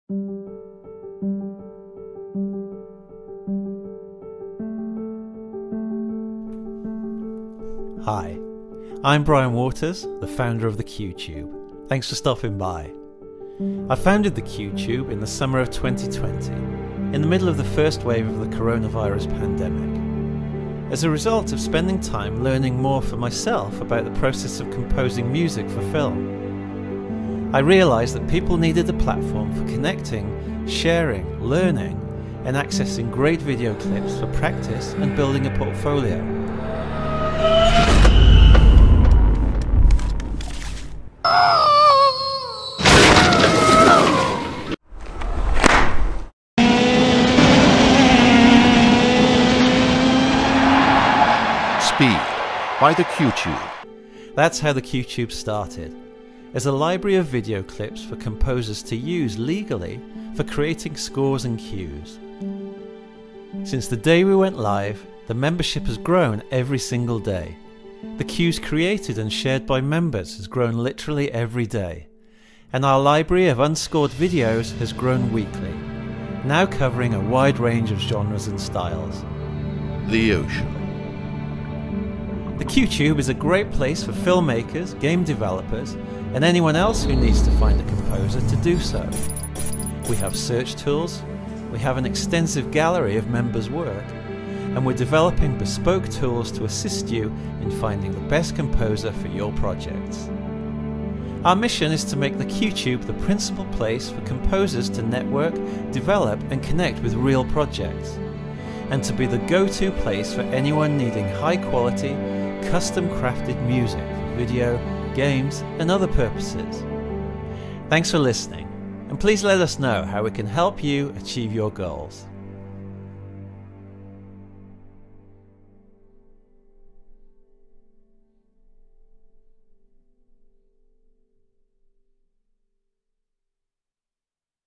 Cue Tube Promo 2020
An introduction to The Cue Tube, a free platform for composers and filmmakers to come together, network, knowledge share, collaborate, build portfolios and get inspired.

Composers can access a free library of legal, unscored clips across a range of genres.

Filmmakers and game producers can find composers for their projects.

Third-party content credits:
Videvo.net
Videvo.net:Beachfront
Videvo.net:stockfootage